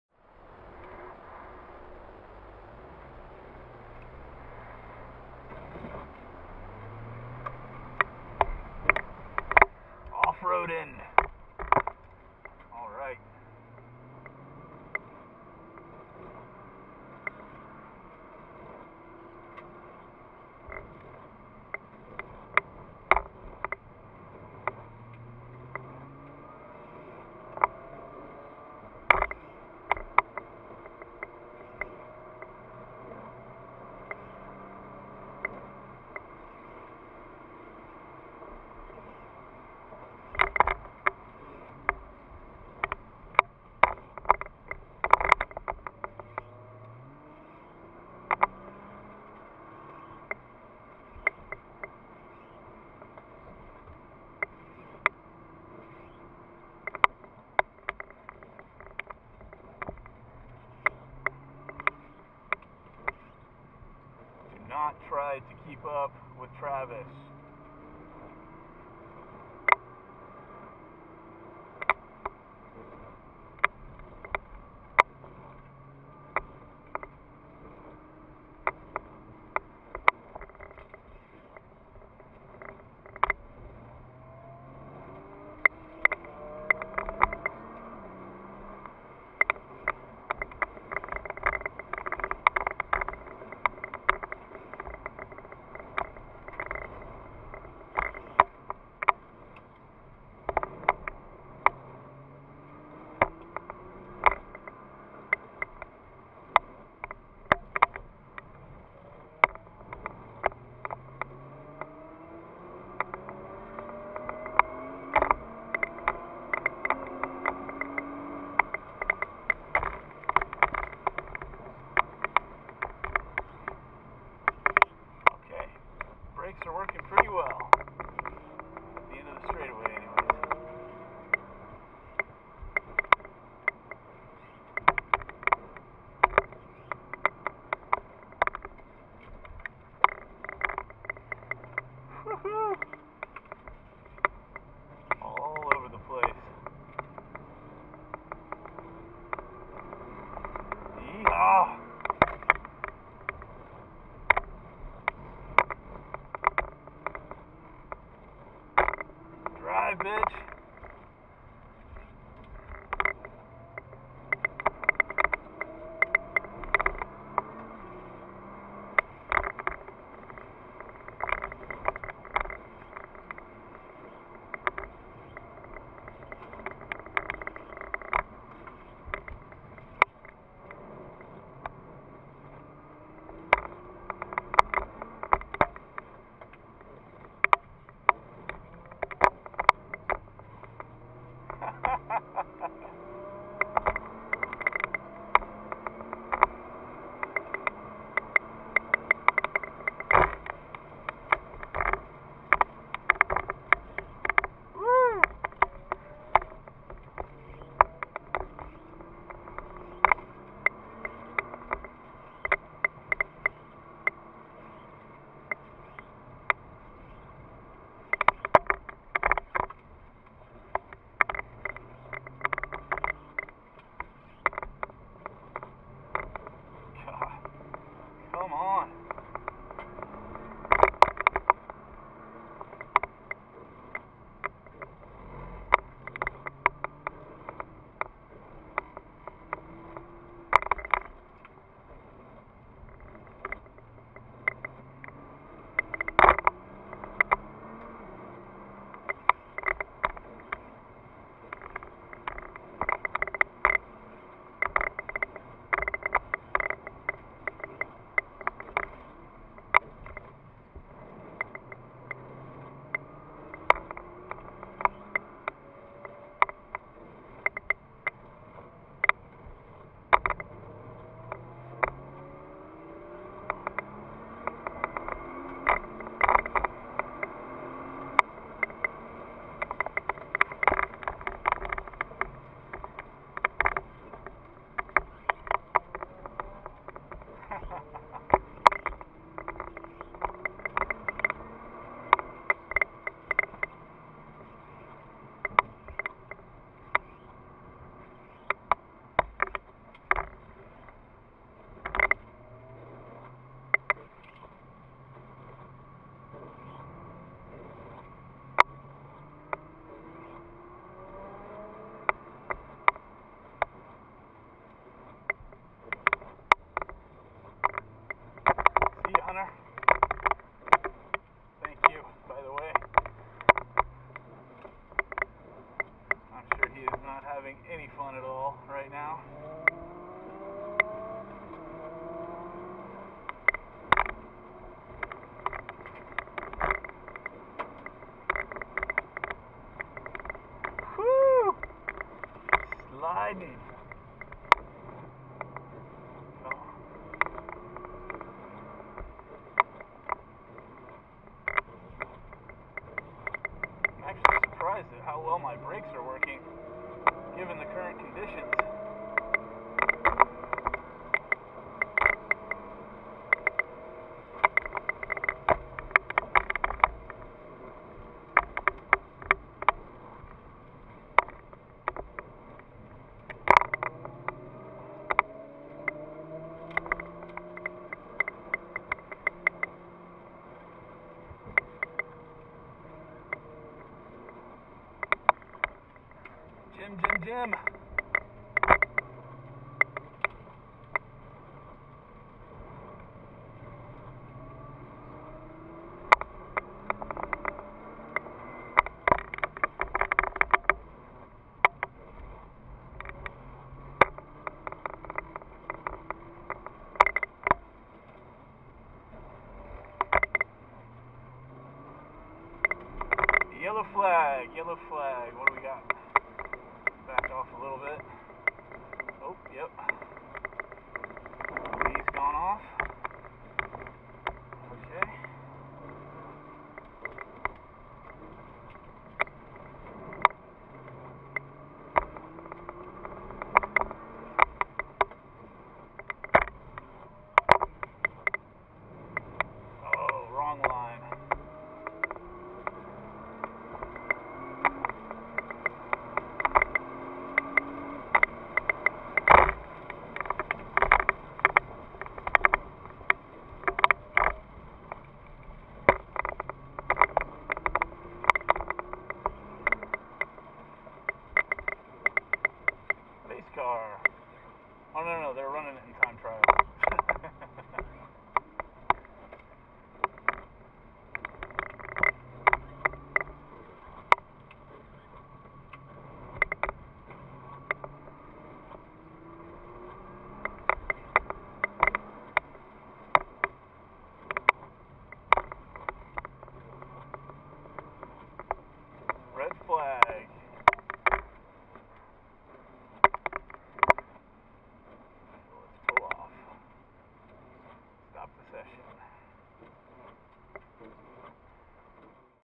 Nissan GT-R - Firebird West In The Rain - NASA-AZ
My first outing in Time Trial with NASA-AZ region. A wet trackday at Firebird West. So much fun sliding around! Yeehaw!